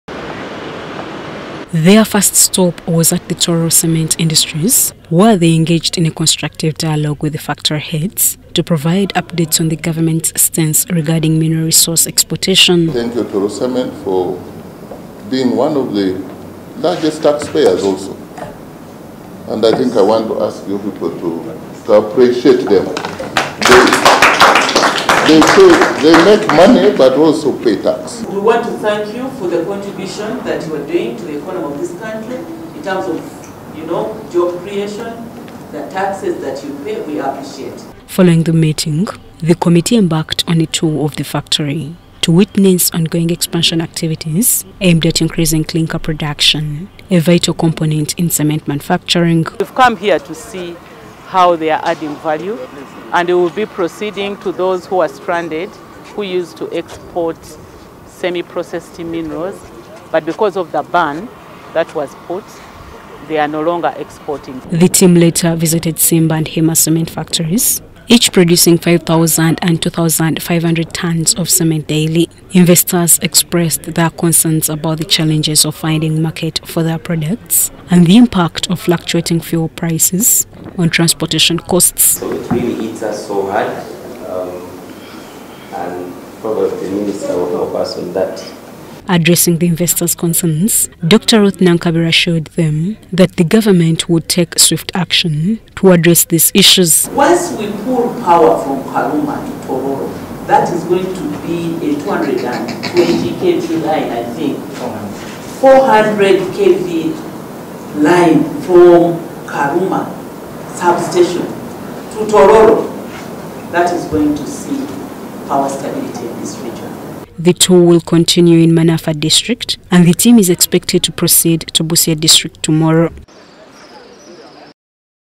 THE CABINET SUBCOMMITTEE ON MINERALS DEVELOPMENT
The Cabinet Subcommittee on Minerals Development, led by Minister of Energy and Mineral Development Dr. Ruth
Manafwa districts.
Nakabirwa, has commenced tour of the Eastern region to assess compliance with the government's regulation on the export of unprocessed mineral resources.
Nankabirwa accompanied by Jacob Oboth Oboth and Peter Lokeris, started the inspection in Tororo and